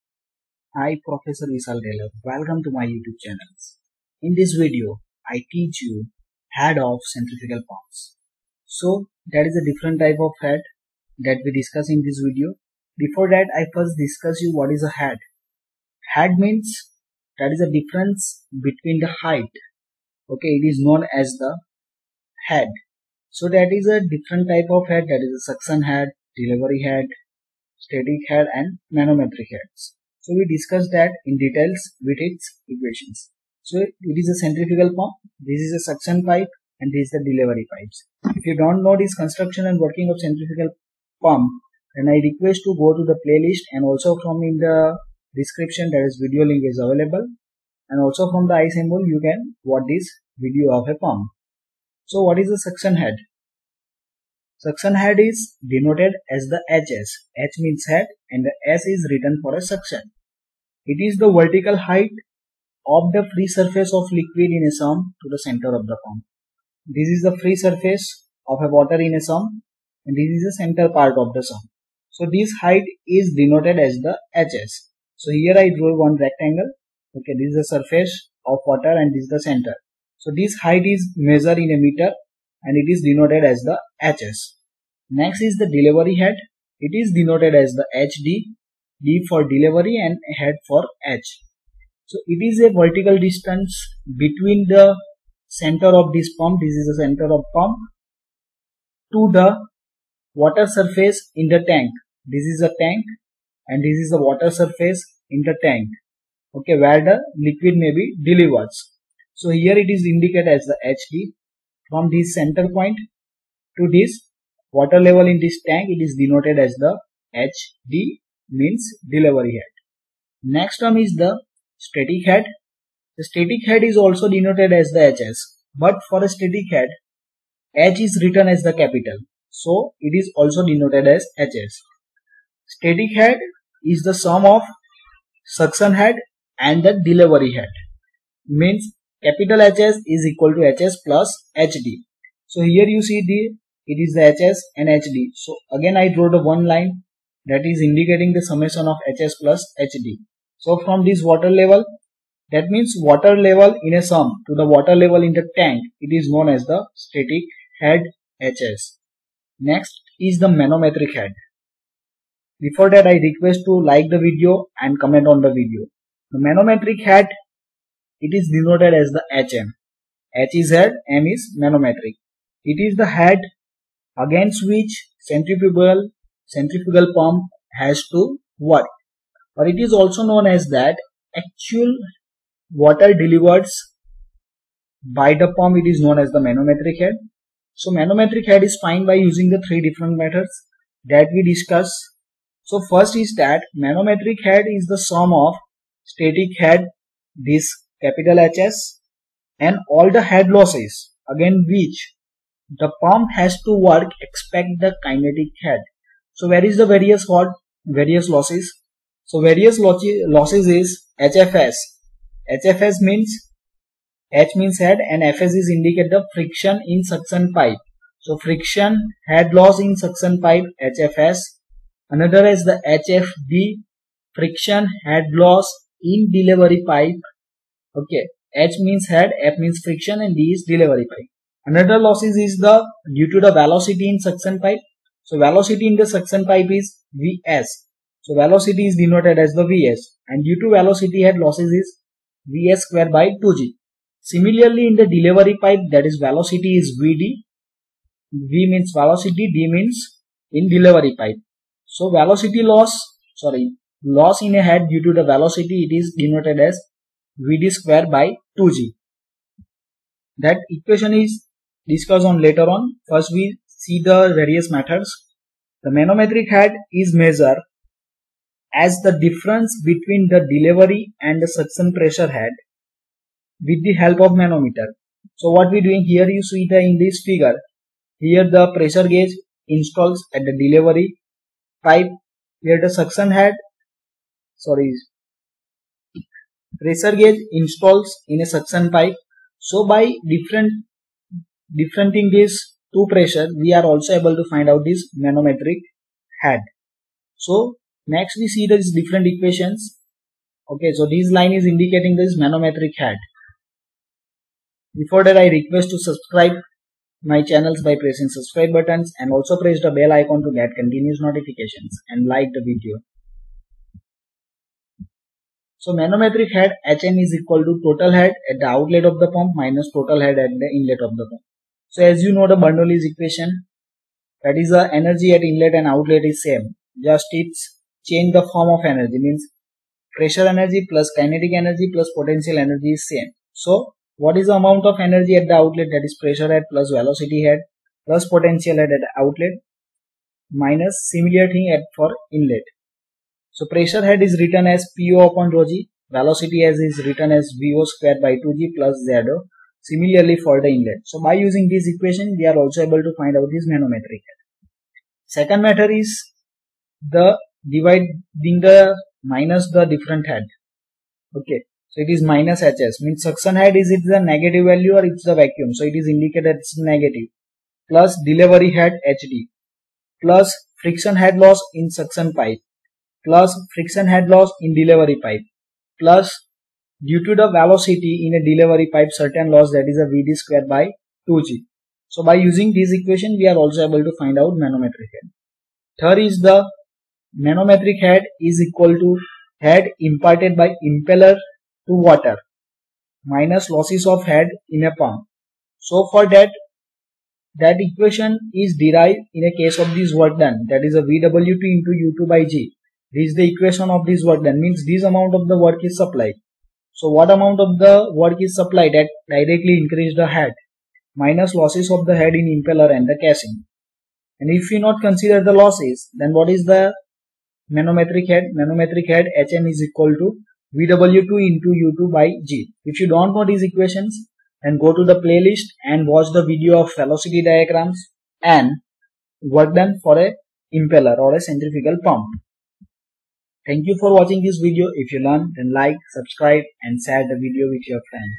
Head of centrifugal pump
In this video, I explained head of centrifugal pump. Following is various type of head.
1. Suction head.
2. Delivery Head.
3. Static Head.
4. Manometric Head.